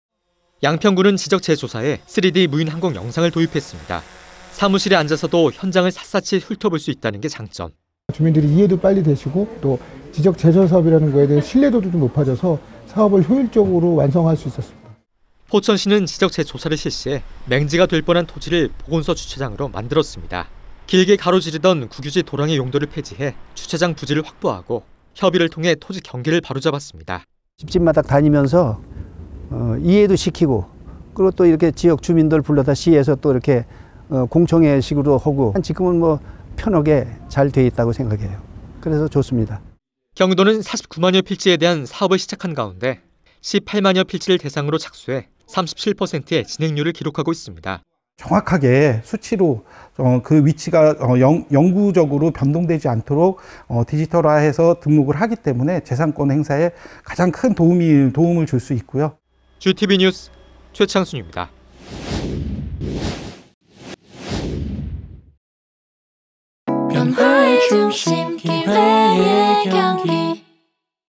[자막뉴스] 경기도 지적재조사에 나타난 드론! 주차장도 만든다고?!
[rep]
양평군은 지적재조사에 3D 무인항공영상을 도입했습니다.

사무실에 앉아서도 현장을 샅샅이 훑어볼 수 있다는 게 장점

[int] 임석호 팀장 / 양평군 지적팀
“주민들이 이해도 빨리 되시고 지적재조사사업에 대해 신뢰도도 높아져서 사업을 효율적으로 완성할 수 있었습니다.”

포천시는 지적재조사를 실시해 맹지가 될 뻔한 토지를 보건소 주차장으로 만들었습니다.

길게 가로지르던 국유지 도랑의 용도를 폐지해 주차장 부지를 확보하고 협의를 통해 토지 경계를 바로잡았습니다.

[int] 신건배 / 포천시 신읍동
“집집마다 다니면서 이해도 시키고 지역 주민들 불러다 시에서 공청회식으로 하고 지금은 편하게 잘 돼 있다고 생각해요. 그래서 좋습니다.”

지적재조사는 2012년 특별법이 제정되면서 시행됐습니다.

경기도는 49만여 필지에 대한 사업을 시작한 가운데 18만 여 필지를 대상으로 착수해 37%의 진행률을 기록하고 있습니다.

[int] 심상현 팀장 / 경기도 지적재조사팀
“정확하게 수치로 그 위치가 영구적으로 변동되지 않도록 디지털화해서 등록을 하기 때문에 재산권 행사에 가장 큰 도움을 줄 수 있고요.”